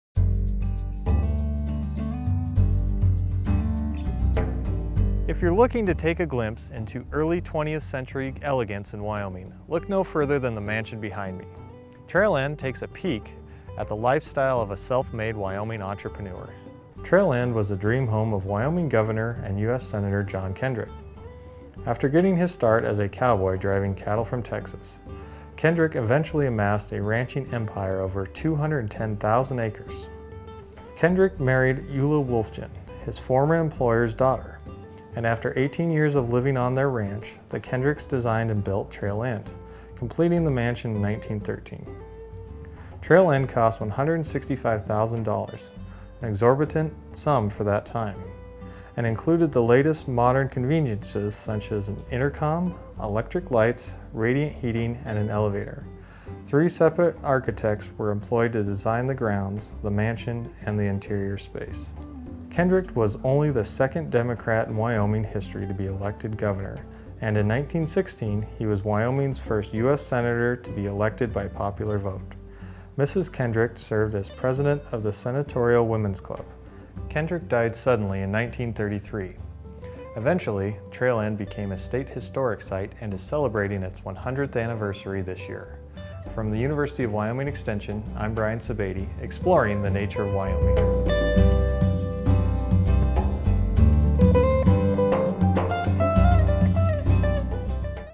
Trail End Mansion
Next time you're driving through Sheridan be sure to stop at Trail End, the former home of Wyoming Governor and Senator John Kendrick.